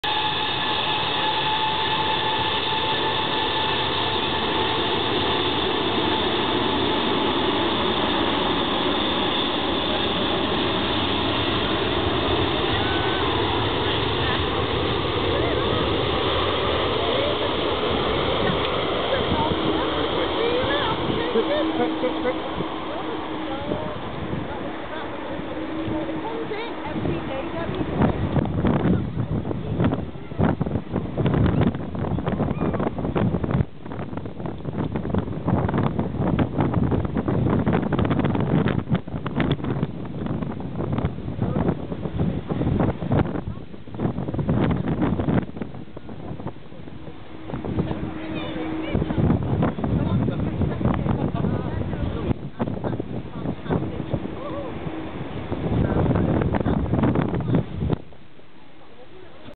take off
emirates airbus a380 taking off into crosswind from manchester international